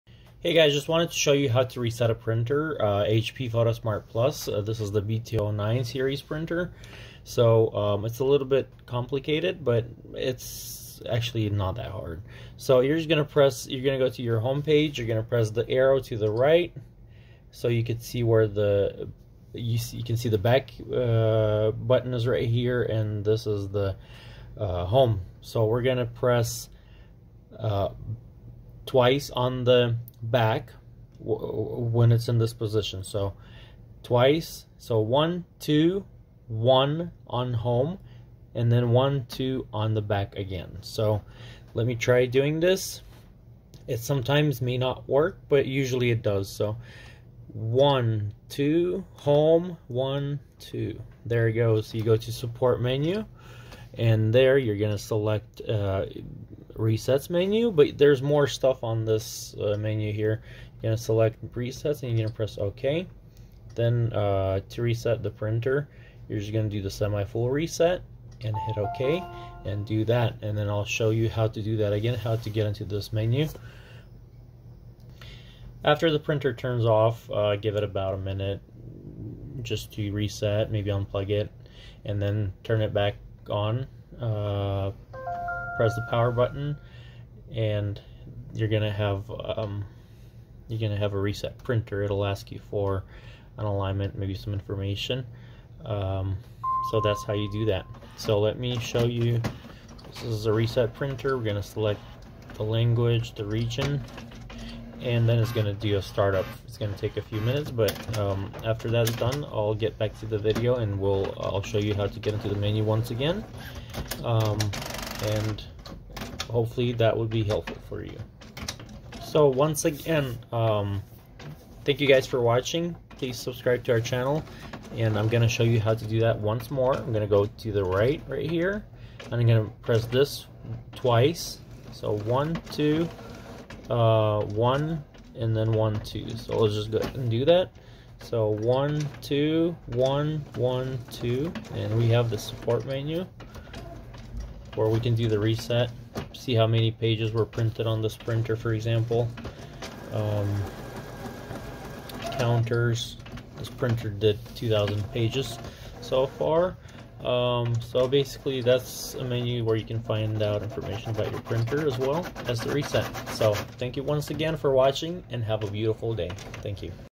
How to Reset HP Photosmart Plus B209a and Access Support Menu on Printer B209 B110a
If you need to reset your printer, follow the directions in the video and you can do that in a few steps.